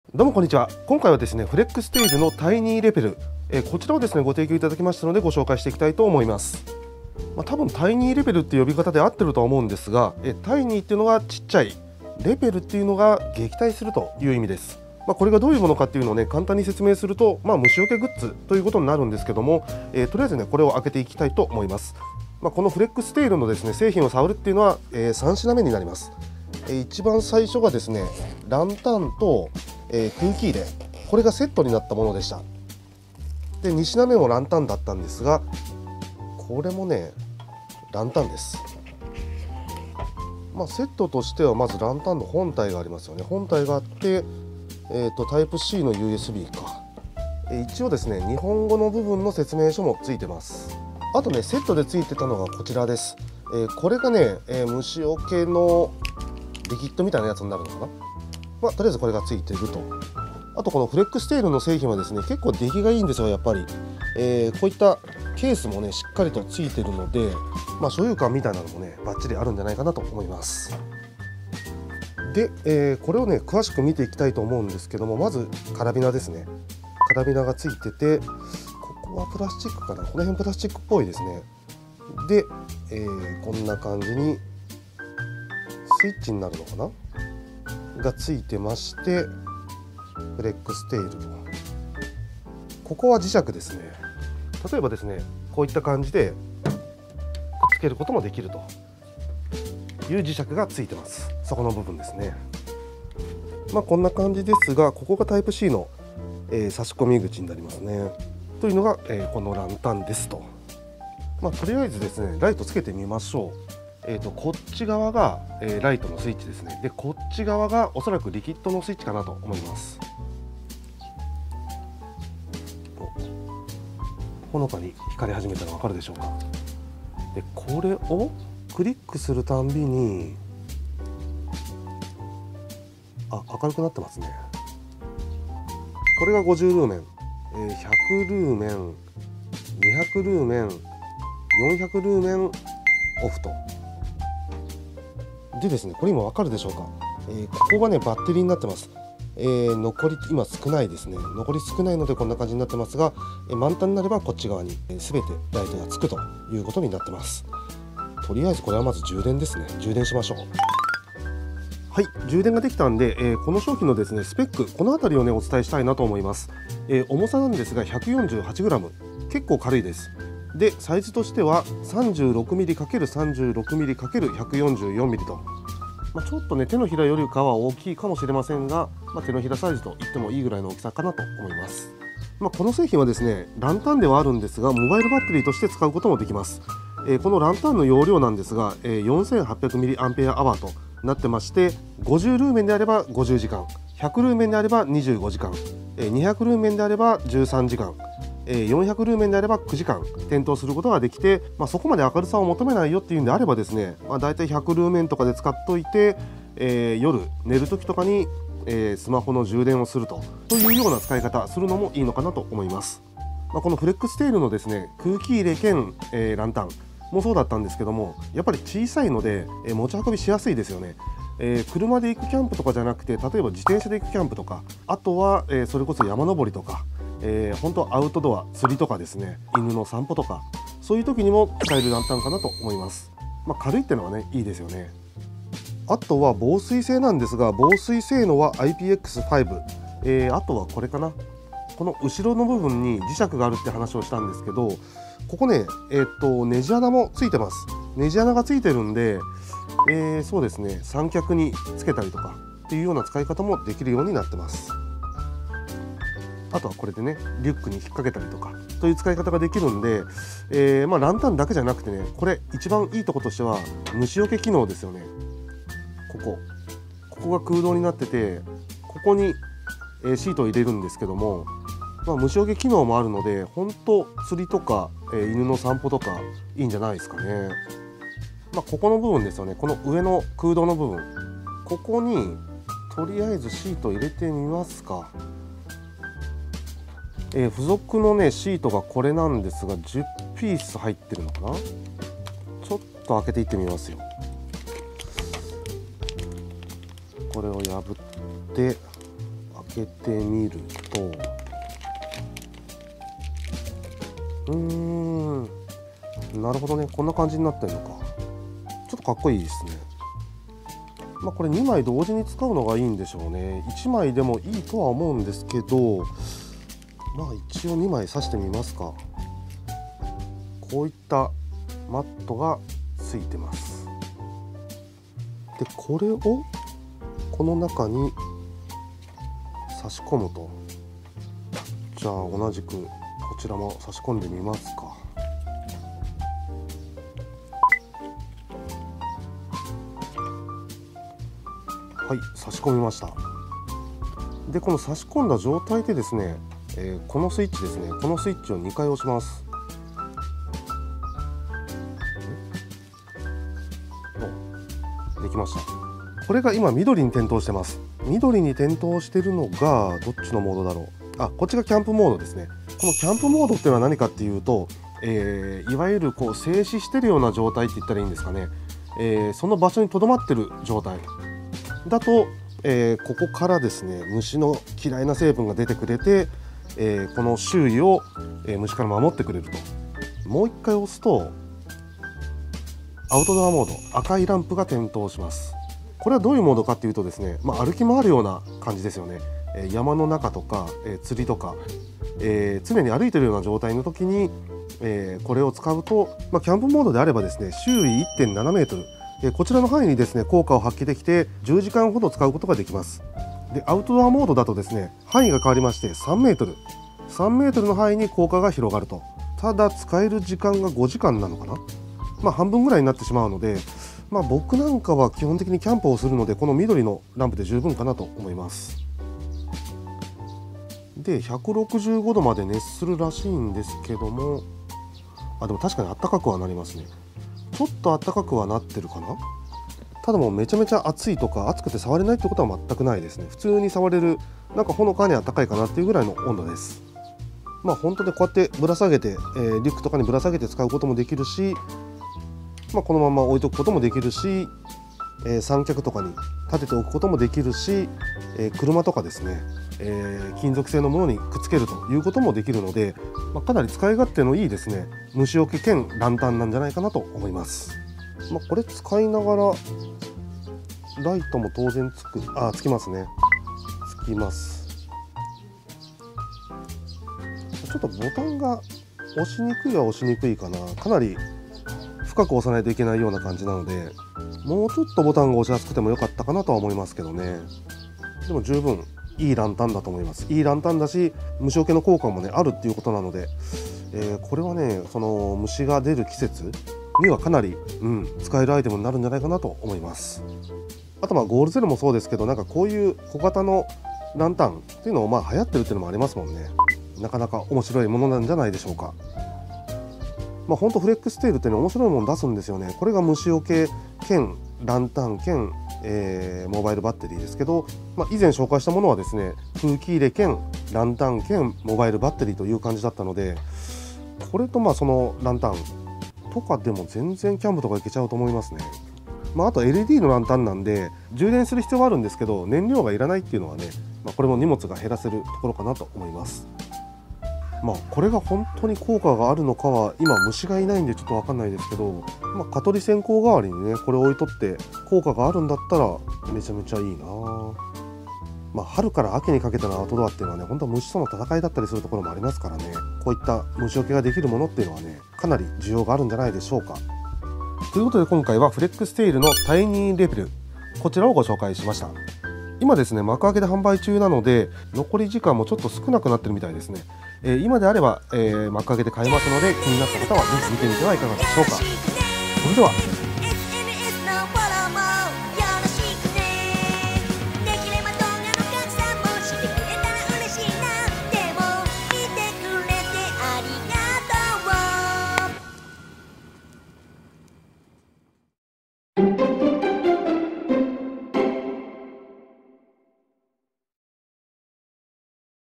キャンプやアウトドア全般に！虫よけ＆ランタン＆モバイルバッテリーの便利グッズ TINY REPEL【FLEXTAIL】
虫に悩まされる夏のアウトドア。
特にプーンと耳元で飛んでくる蚊は困りものです。
今回紹介するTINY REPEL【FLEXTAIL】は、虫よけだけでなくランタンでもありモバイルバッテリーでもあるという1台3役の優れもの。
静止状態で使うキャンプモードと動くシーンで使うアウトドアモードの2種類があり、色んなシチュエーションで使えます。
キャンプ・釣り・犬の散歩・登山…など、外遊びが好きな人にとって良い物じゃないでしょうか。
ユスリカと蜂で実験したようですが、ヌカカにも効いたりしたらいいなぁ…。あの痒さは忘れられません。

愛用中のおすすめキャンプギアをまとめてます

↓アウトドア中華鍋 スケーターオンラインショップ

↓スケーター×かの地から 深型メスティン

↓【しーまんデザイン】8～10号まで入る限定オピネルナイフケース